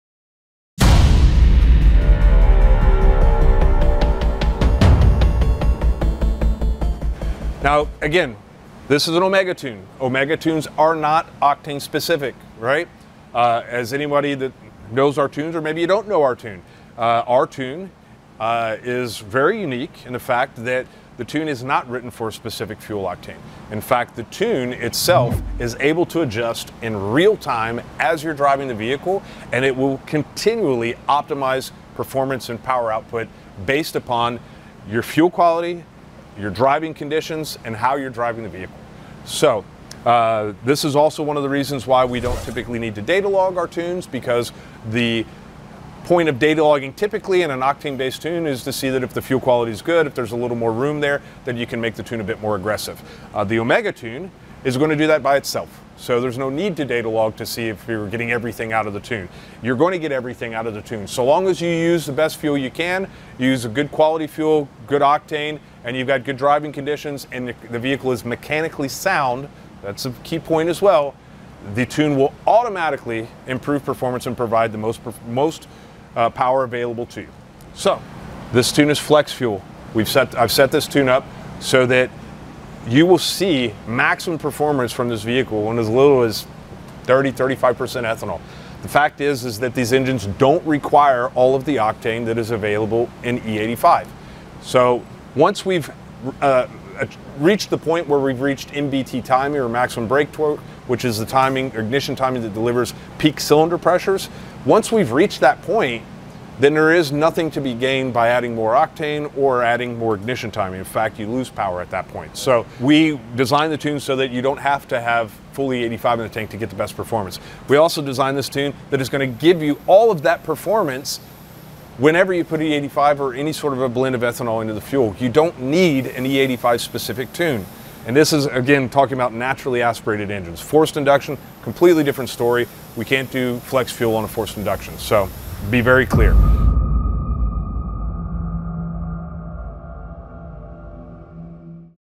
Oz Tuning Omega tune - How it works!
A brief description on how the Oz Tuning Omega tune, designed by Ken Osborne works. Excerpts taken from a recent video.